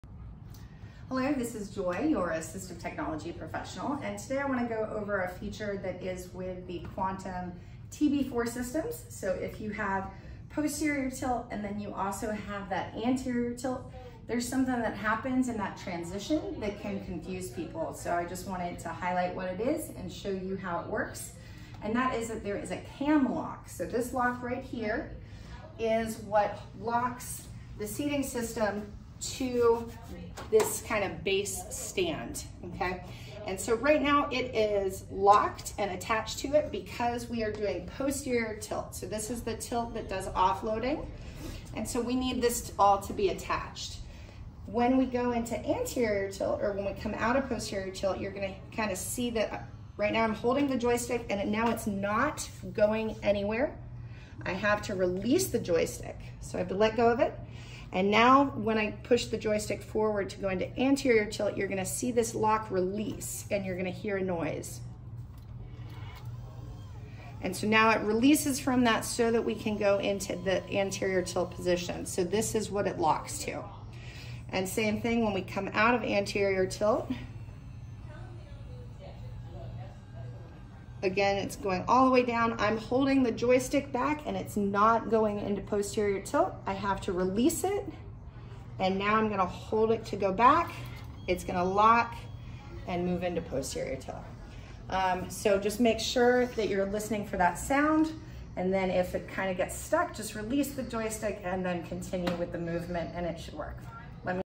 CAM locks for Anterior/Posterior Tilt on Power Wheelchair
On some of the new chairs, there is a cam lock. This lock has to change positions when switching between power anterior and posterior tilt on the power wheelchair.

When the lock switch is, there is a noise that you will hear. There is also an action required with the joystick to engage in that opposite direction of movement.

This video explains that and shows how that it works.